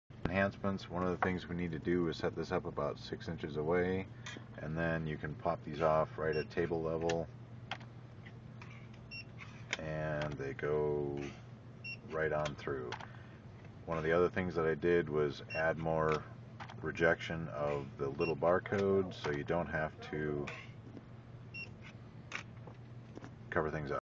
E-Form AutoFill
Demonstration of the Code CR1000 scanning driver's licenses with ease and parsing the data - with all AAMVA Compliant DLs.